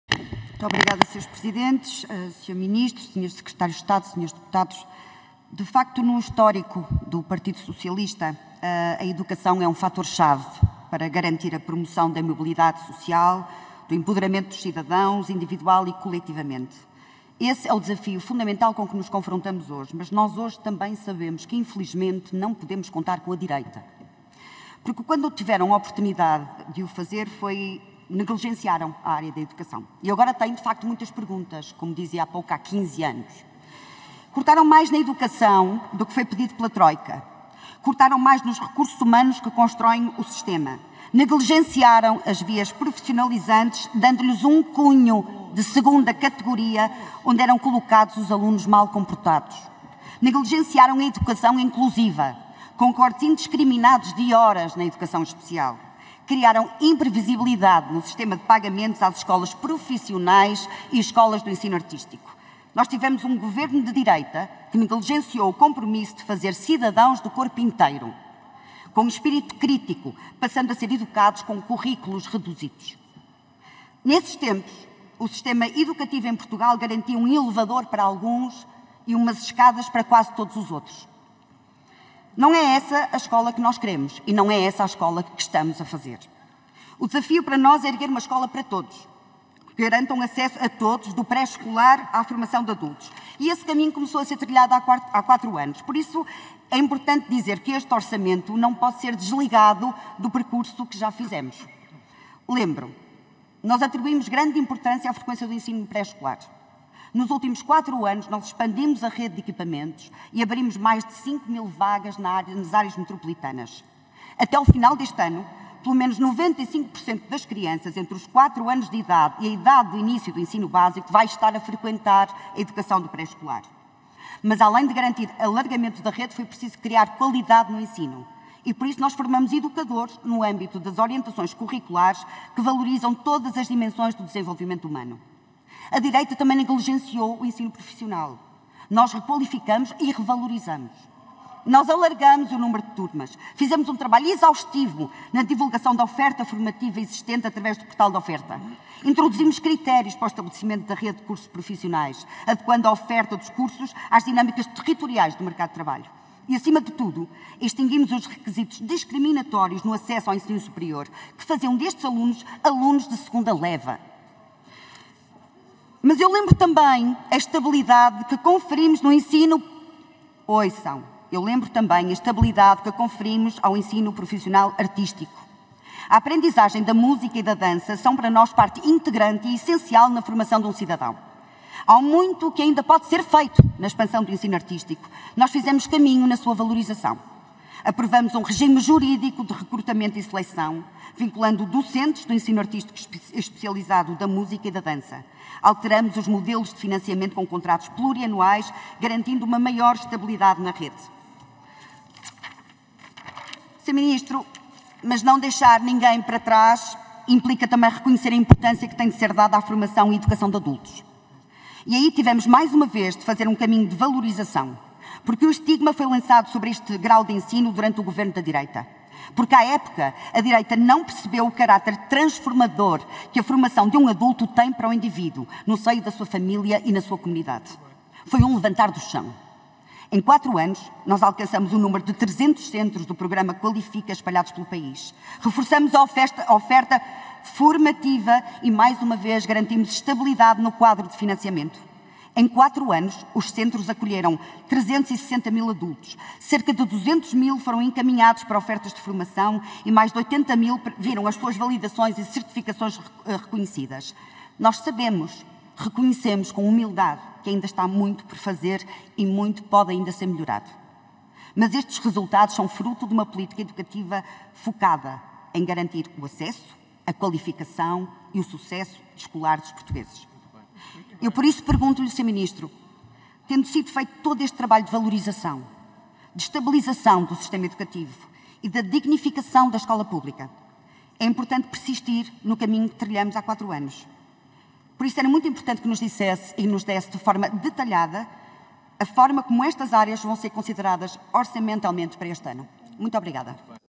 17-01-2020 | Debate OE2020 - Audição do Ministro da Educação | Carla Sousa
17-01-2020 | Intervenção da Deputada Carla Sousa - Audição do Ministro da Educação, Tiago Brandão Rodrigues - Debate na Especialidade do Orçamento do Estado para 2020 e Grandes Opções do Plano para 2020 - Comissão de Orçamento e Finanças - Comissão de Educação, Ciência, Juventude e Desporto